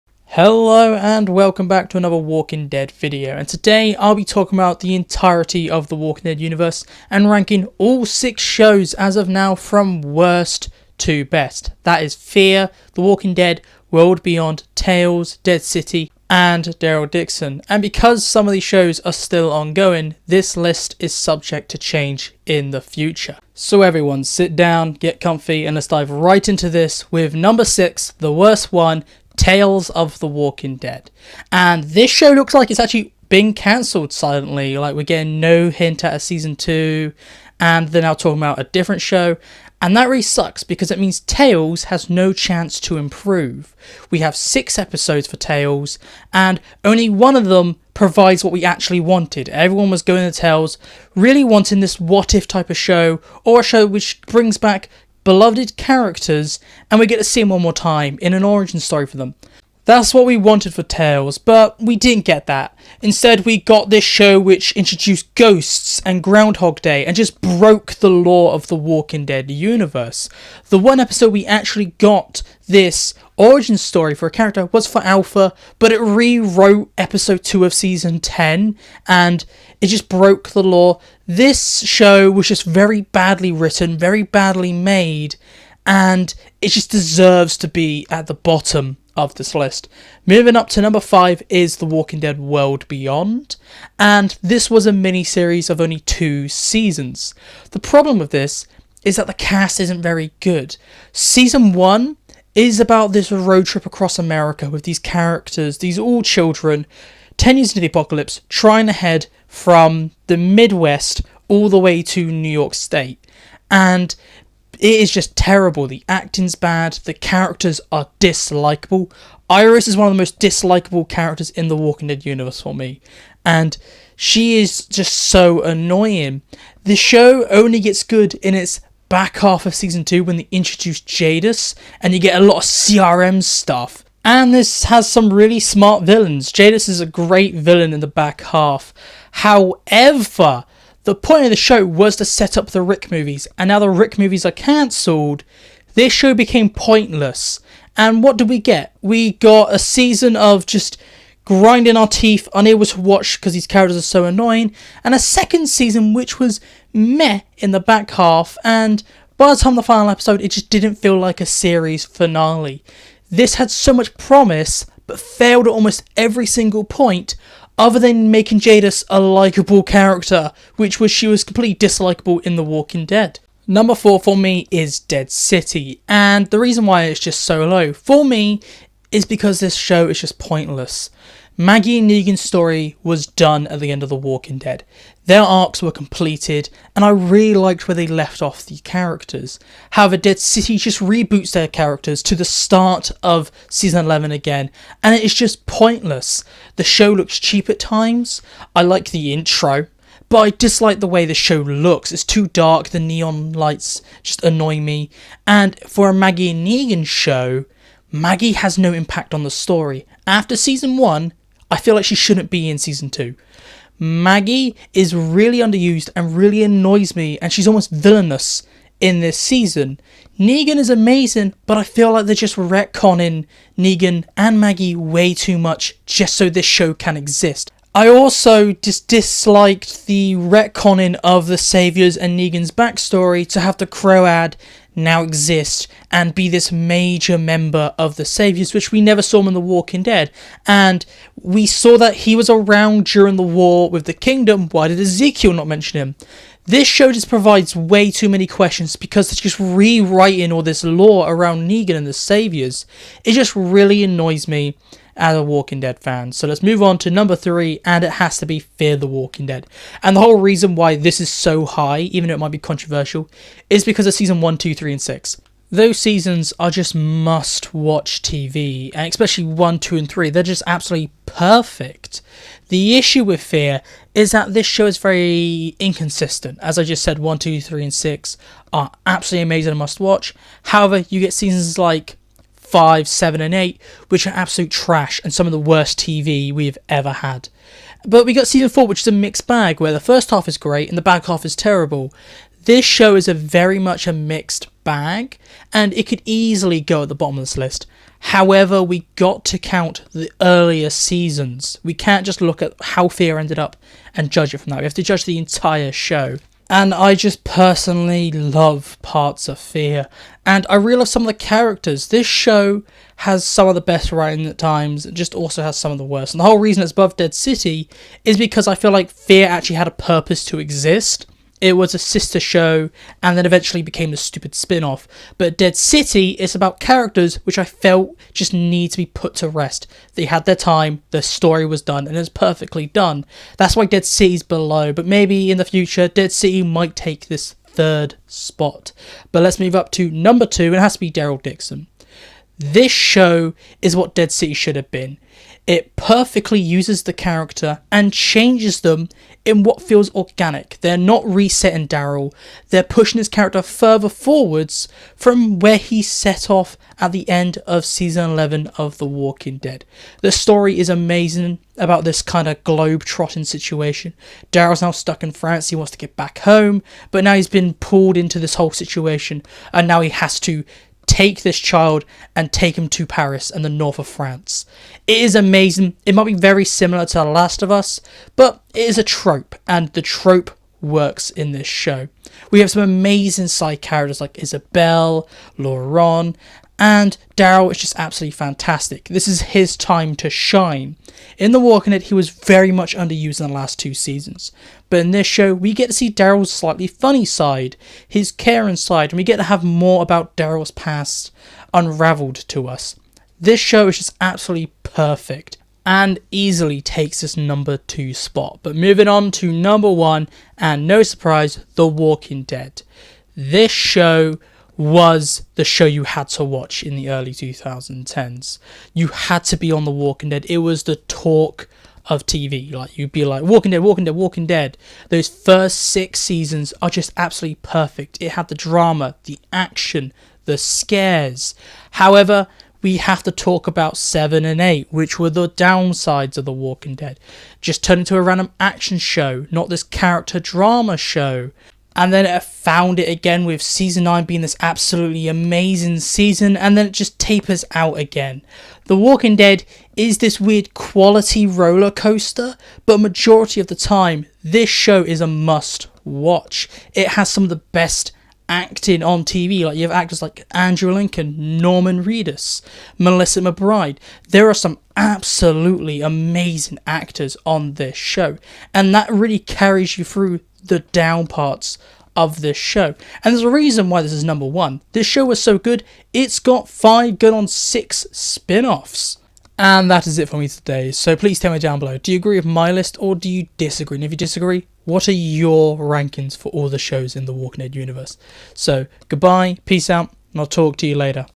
The Walking Dead Universe All Shows Ranked Worst To Best
Every Show In The Walking Dead Universe Ranked Worst To Best. In this video I will be looking at The Walking Dead , Fear The Walking Dead, The Walking Dead World Beyond, Tales Of The Walking Dead, The Walking Dead Dead City and The Walking Dead Daryl Dixon. Then ranking show and breaking down why I like or dislike each show looking at both the good and bad of each show and their respective seasons.

Remember this is just my personal opinion on the worst to best seasons, so if you have a different list make sure to tell me down in the comments.